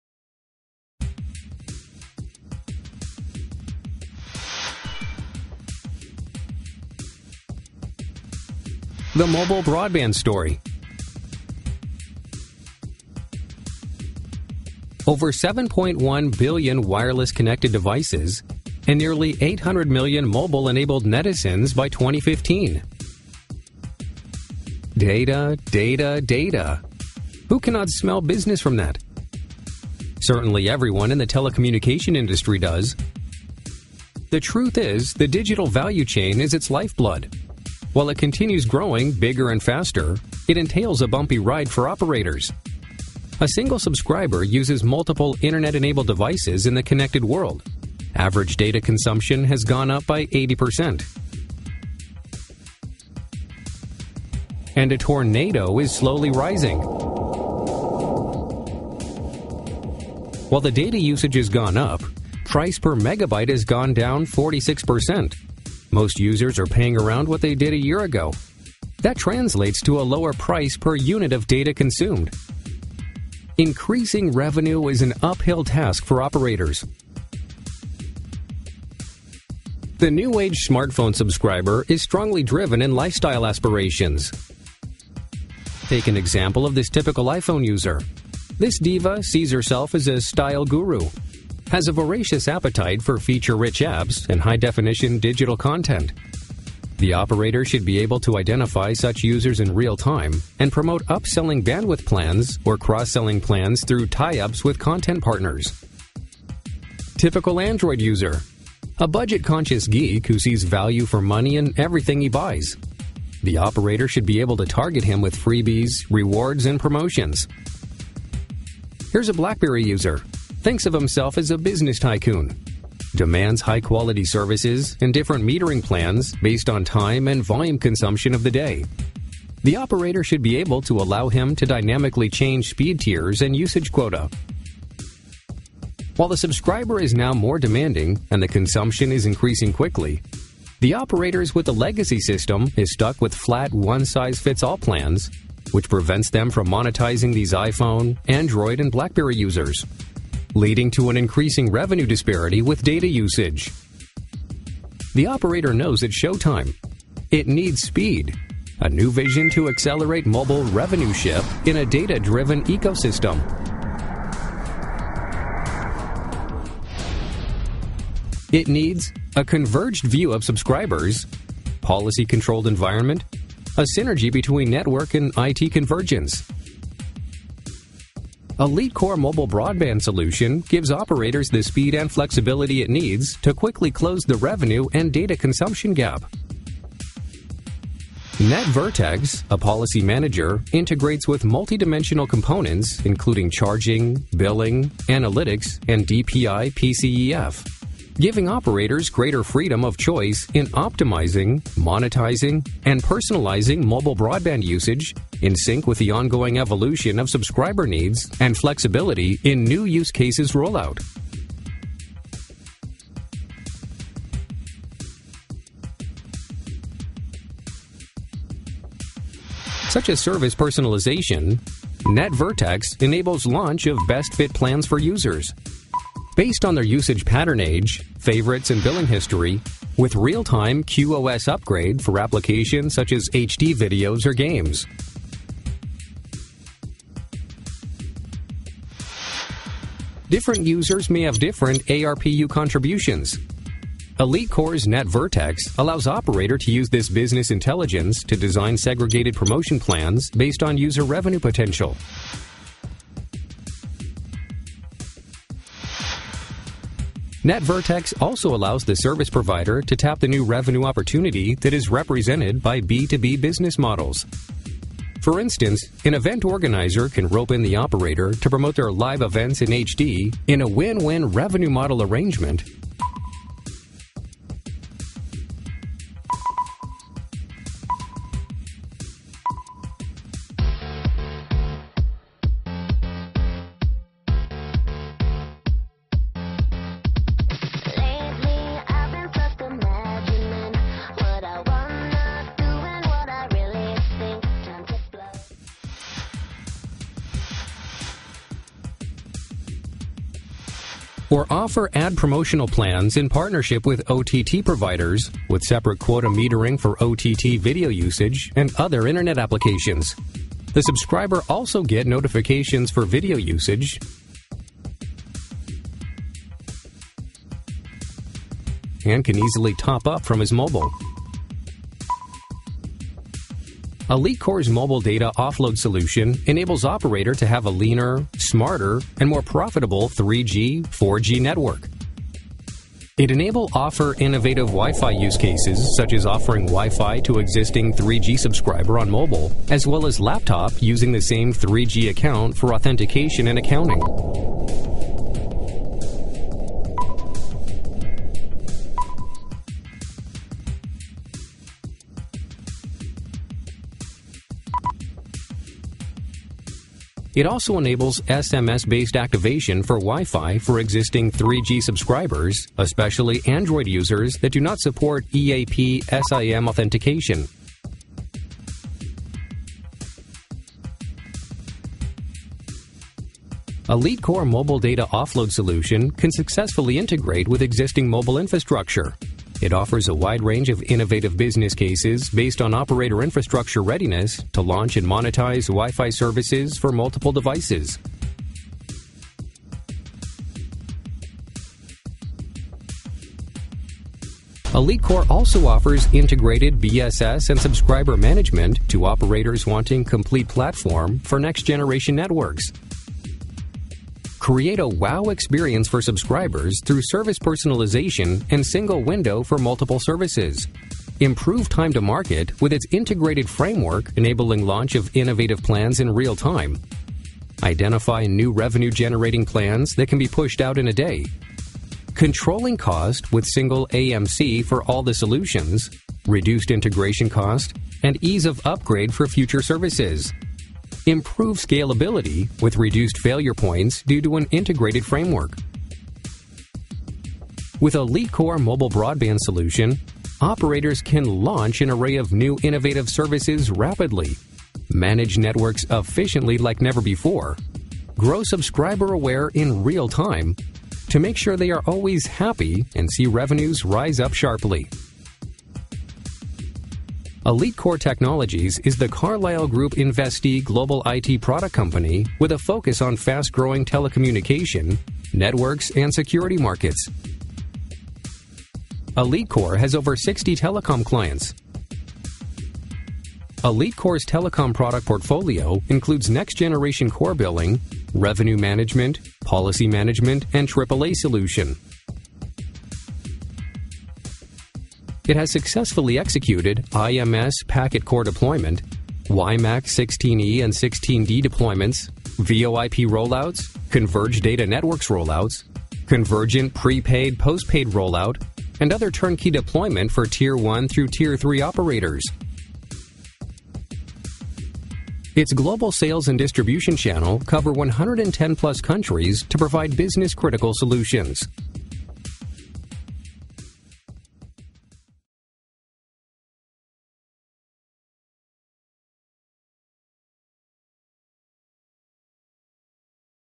The Mobile Broadband Story
The ongoing 3G-4G evolution is creating revenue threats for service providers as they struggle to find ways in accommodating the growing data appetite of next-generation smartphone users while trying to monetize the same.

Enjoy this thrilling movie where you put yourself in the operator's driving seat, trying to manage the data overflow across your limited 3G-4G spectrum.

Mind you, it's going to be an uphill task with subscribers dictating terms in the data eco-system but with over 7.1 billion wireless connected devices and nearly 800 million mobile-enabled netizens by 2015, the stakes are high and the reward is huge!

As you walk half-way in the operator's shoes trying to close the data-revenue gap, you'll meet a FRIEND -  a friend who helps you accelerate revenues with innovative offerings, manage networks efficiently, grow subscriber-aware in real time and unearth what really happens in the network.